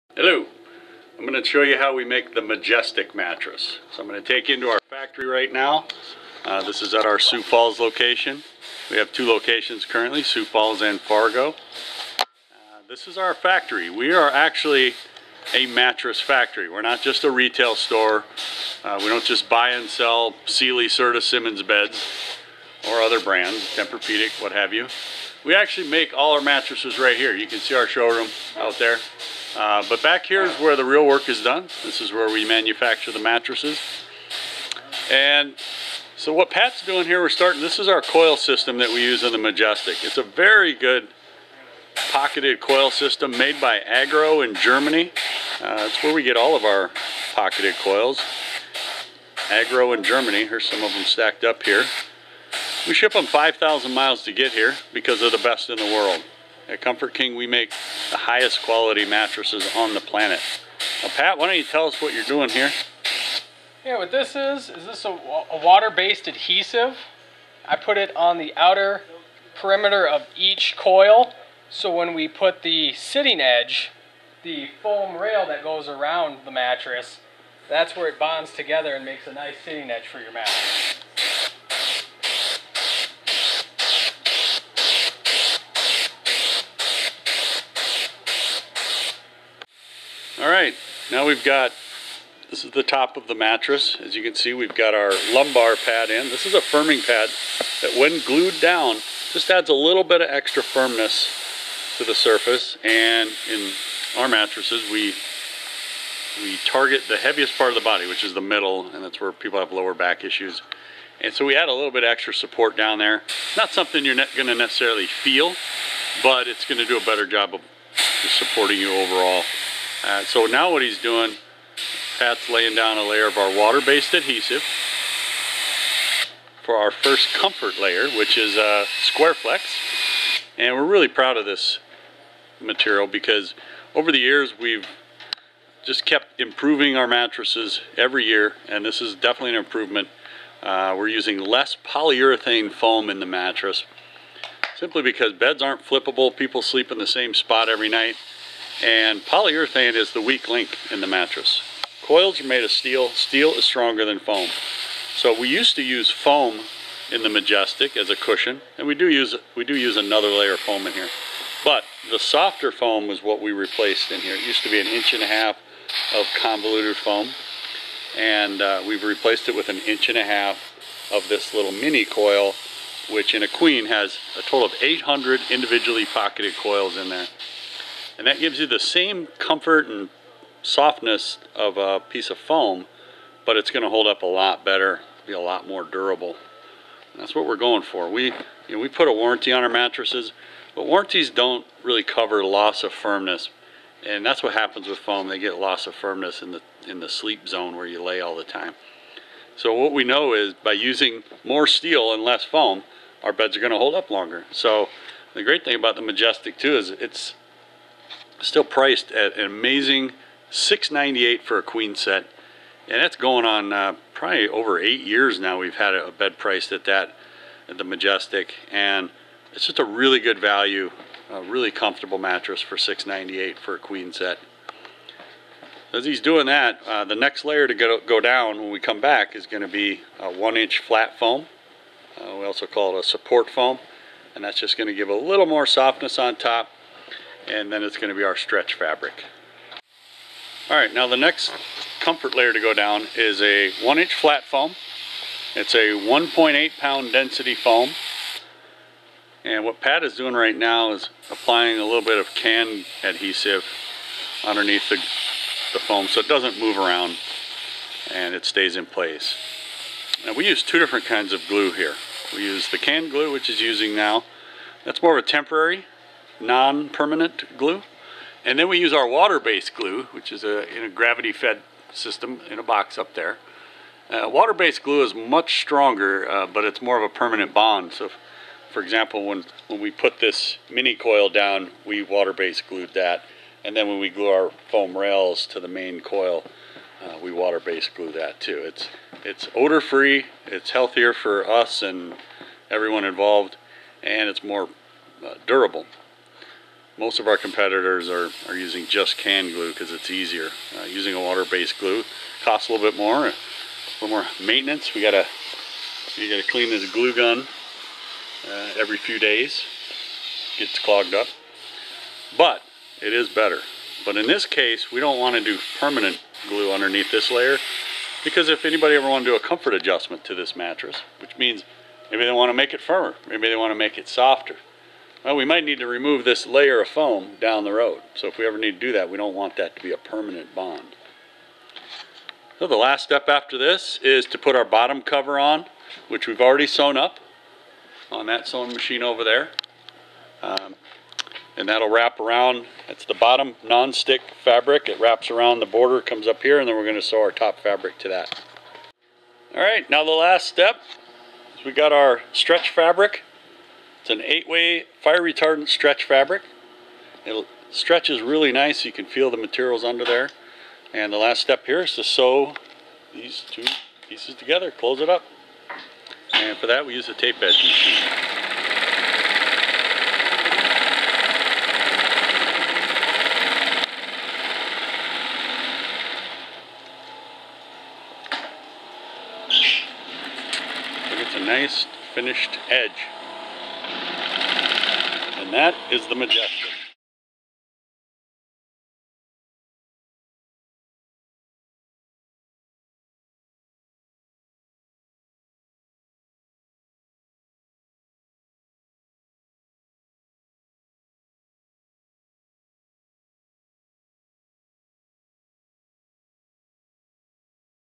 Majestic 2017
The MAJESTIC is everything (& more!) that the name implies.
Actually, it's not thaaat majestic, it's merely a really comfortable "Goldilocks" feeling (not too firm or too soft) mattress for the sweet low price of $698 for the queen set. Watch the video. You'll see how great it is.